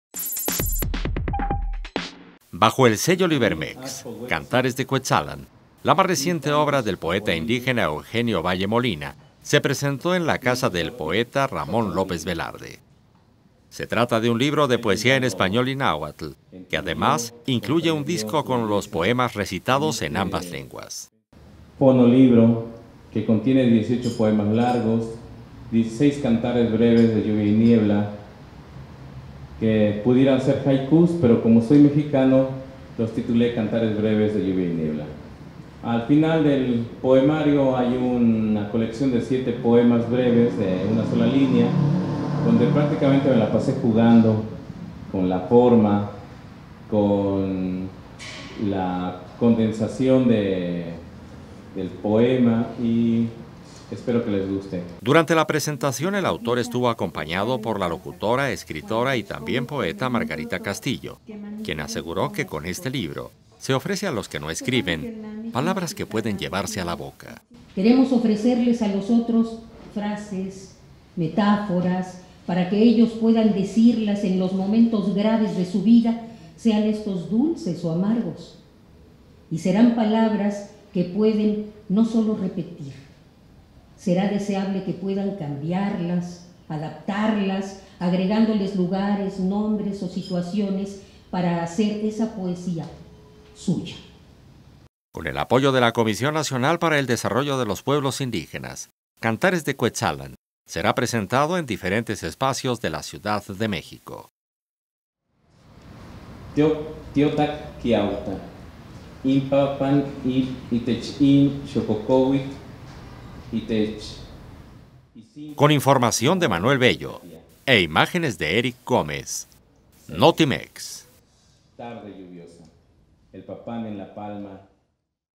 Presenta Eugenio Valle Molina "Cantares de Cuetzalan"
Se trata de un poemario en náhuatl y español que incluye un fonolibro